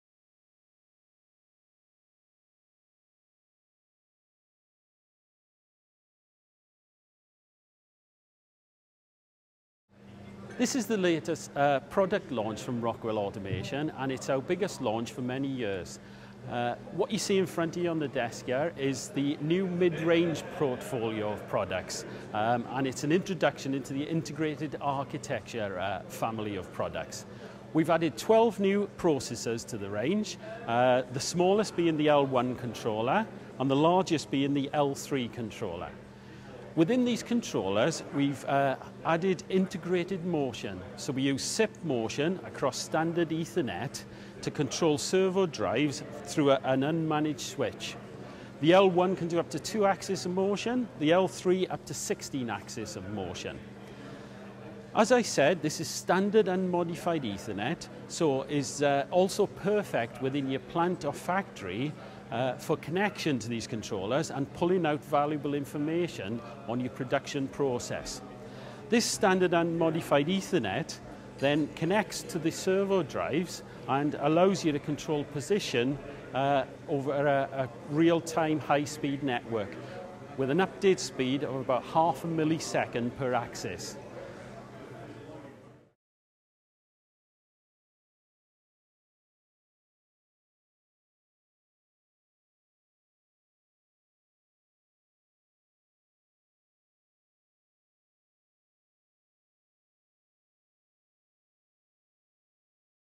Allen Bradley - New CompactLogix 5370 / Kinetix 350 at Manufacturing Automation 2012, Ireland
Rockwell Automation Ireland introduce the new Allen Bradley Mid Range Architecture at Manufacturing Automation 2012. This event was hosted by Hanley Automation in May 2012. Here we introduce the new Compactlogix 5370 controller family, the new Panelview Plus 6, and Kinetix 350 servo drives with CIP servo motion control over Ethernet/IP